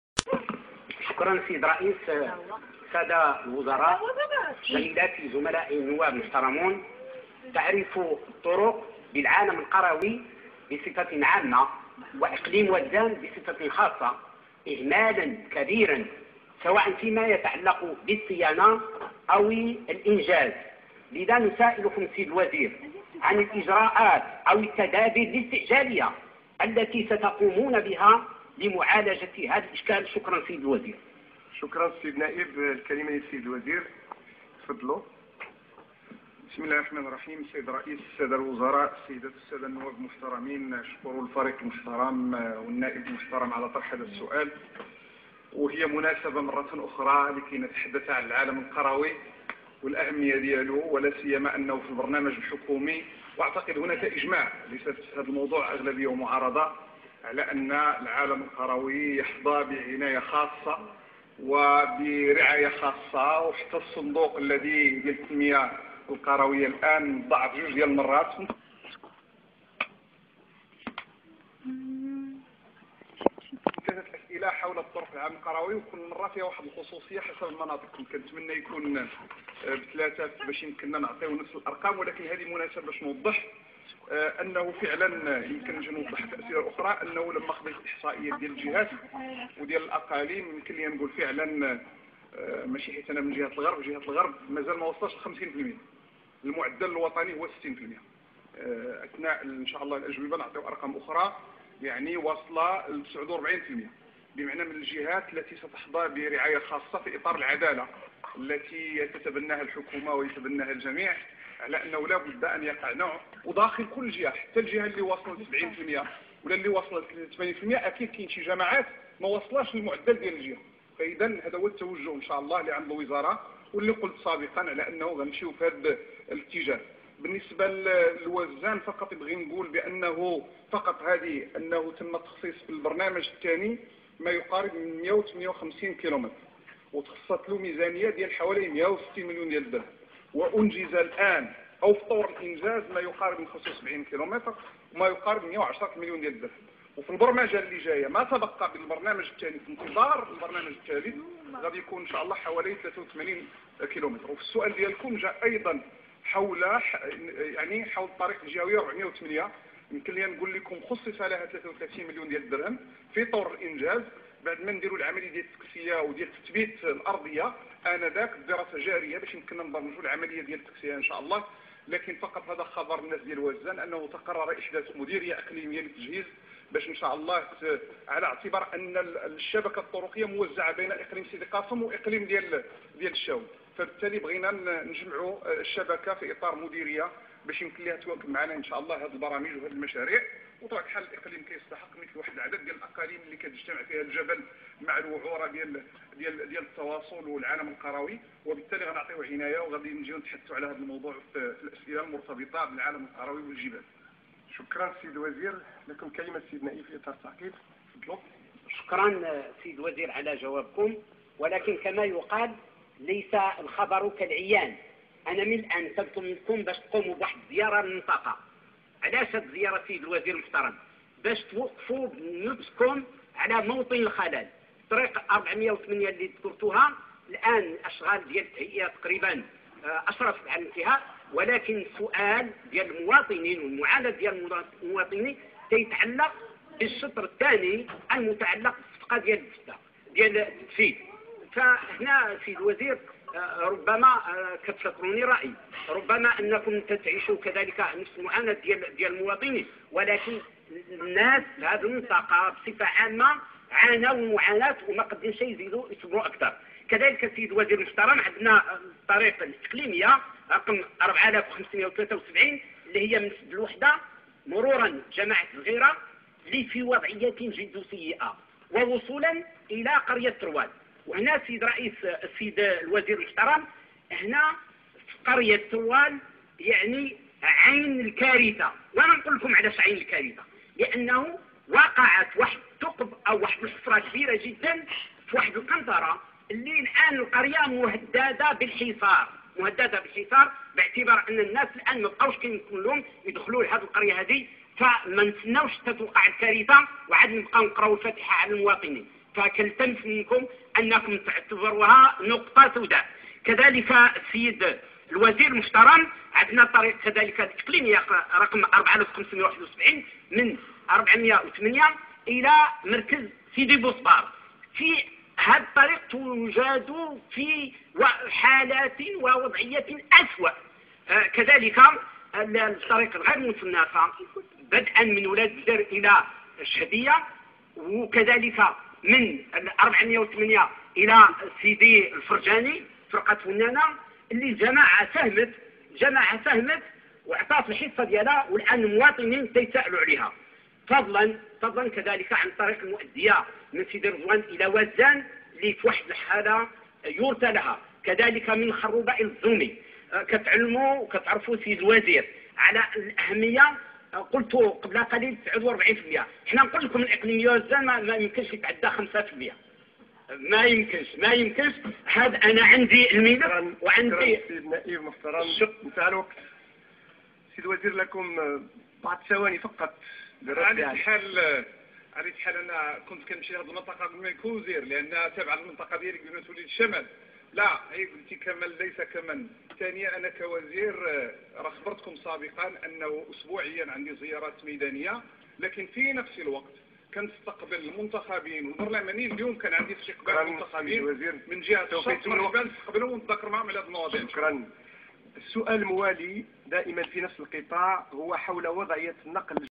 محمد الحجوجي يتحدث عن مشاكل النقل في وزان
سؤال للنائب البرلماني محمد حجوجي زوال يوم الاثنين 30 ابريل 2012م.